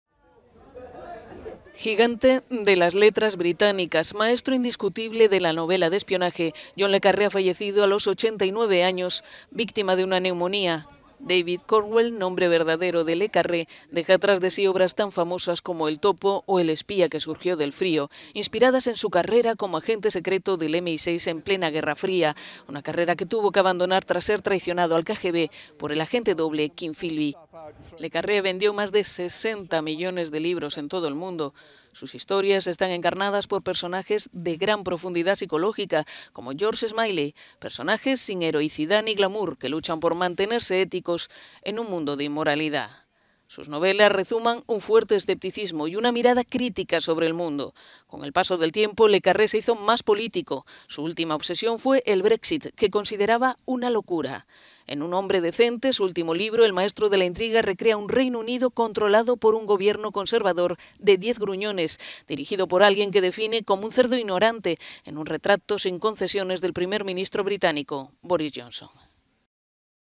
Muere John Le Carré, el escritor que se inspiró en sus vivencias como espía para sus novelas
David John Cornwell, su verdadero nombre, consiguió que la novela de espías trascendiese el género y la elevó a niveles que nunca antes había conocido describiendo los tiempos de la Guerra Fría